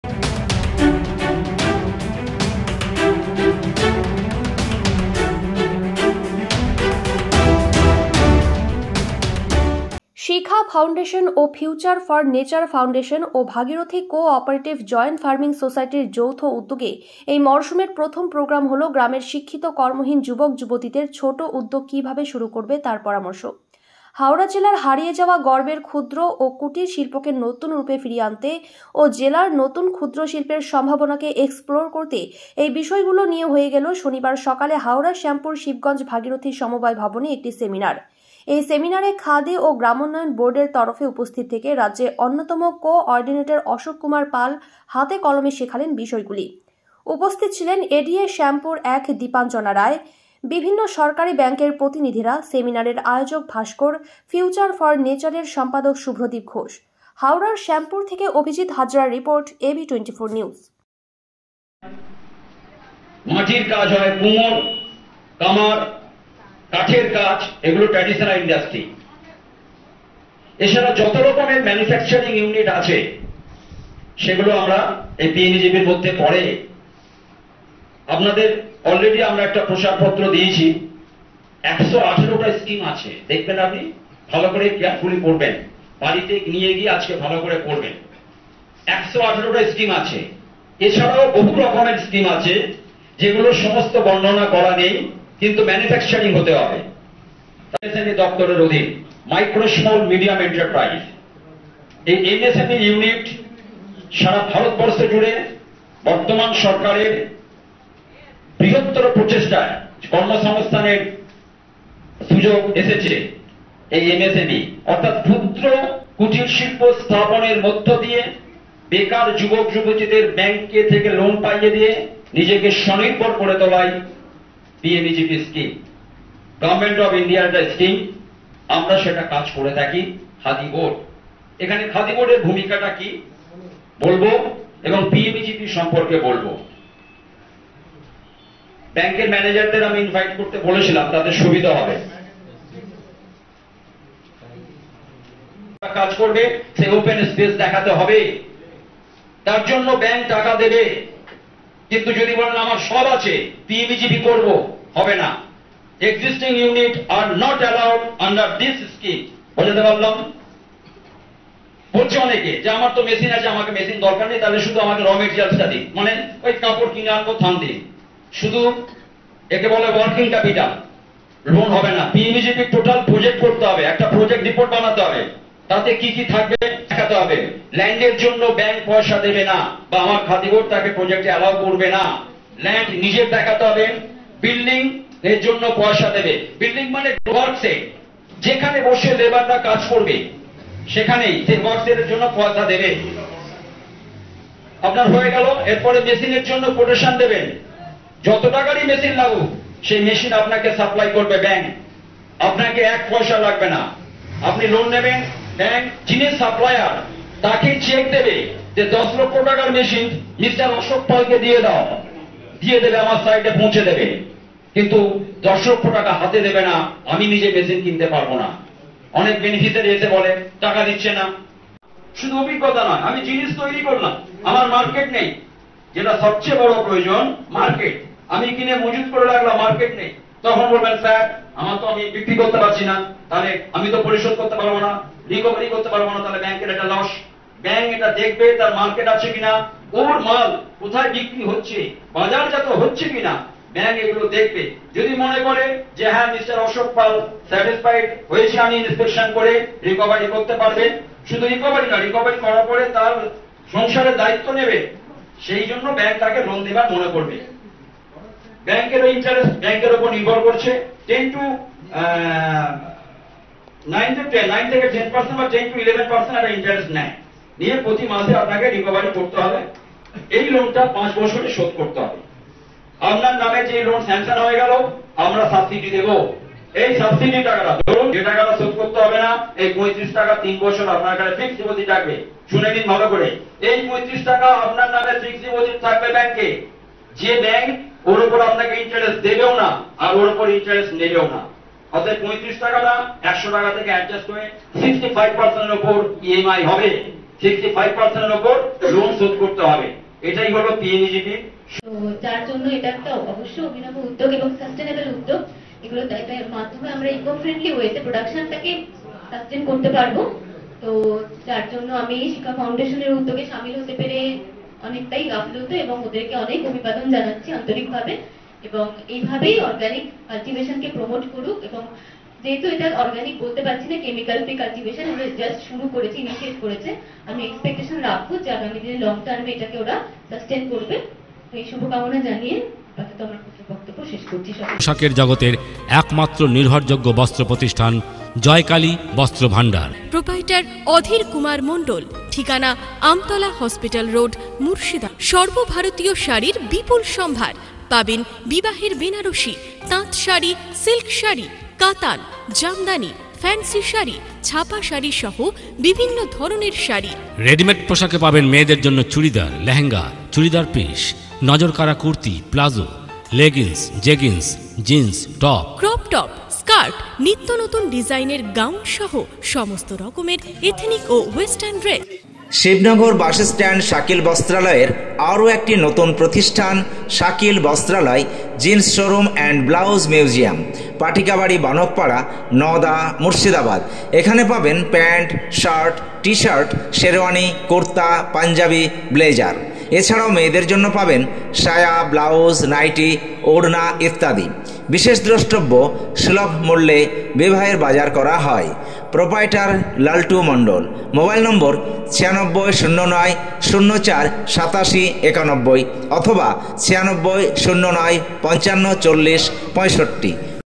ক্ষুদ্র কুটির শিল্পকে নতুন রুপে ফিরিয়ে আনতে।।শিক্ষীত কর্মহীন যুবক যুবতীদের নিয়ে কর্মশালা।।হাওড়া।।
WORKSHOP WITH EDUCATED UNEMPLOYED YOUTHS TO REVIVE SMALL AND COTTAGE INDUSTRIES   ক্ষুদ্র কুটির শিল্পকে নতুন রুপে ফিরিয়ে আনতে।।শিক্ষীত কর্মহীন যুবক যুবতীদের নিয়ে কর্মশালা।।হাওড়া।।